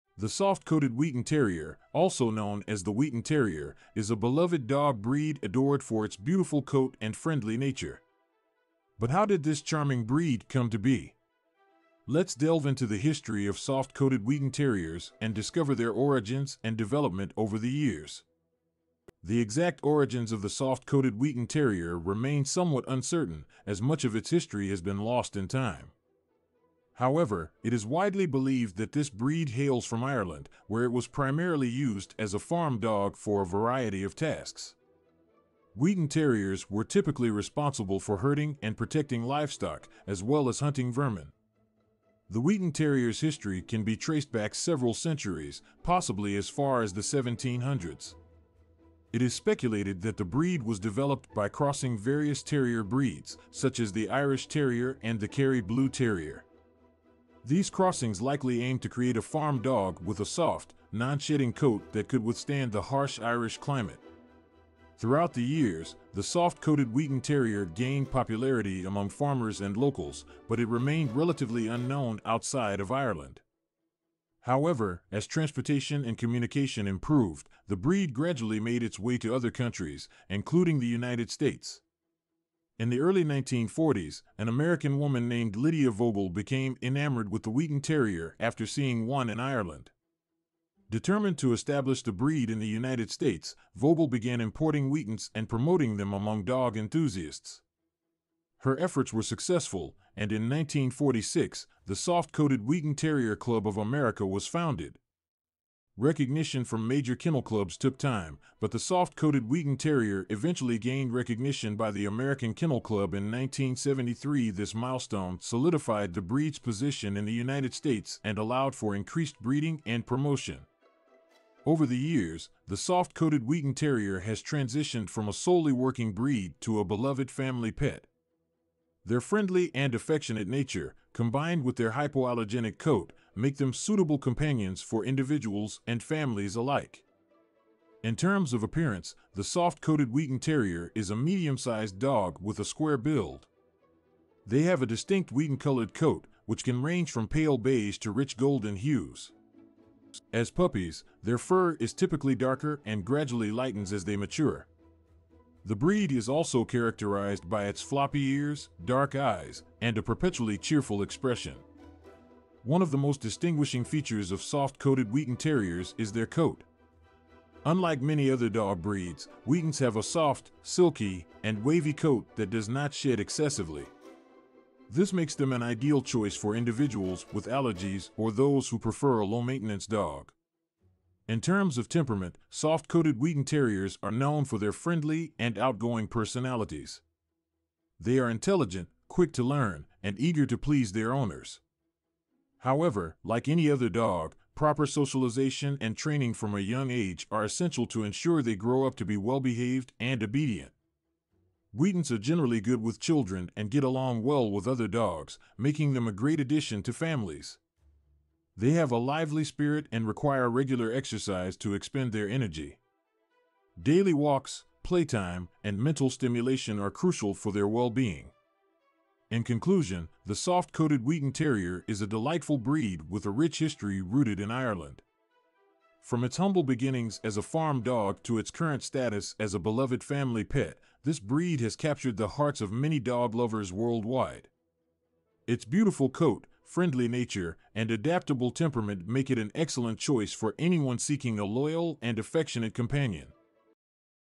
Everything You Need to Know About Soft Coated Wheaten Terriers
The Soft Coated Wheaten Terrier, also known as the Wheaten Terrier, is a beloved dog breed adored for its beautiful coat and friendly nature
 But how did this charming breed come to be? Let's delve into the history of Soft Coated Wheaten Terriers and discover their origins and development over the years
The exact origins of the Soft Coated Wheaten Terrier remain somewhat uncertain, as much of its history has been lost in time
 However, it is widely believed that this breed hails from Ireland, wh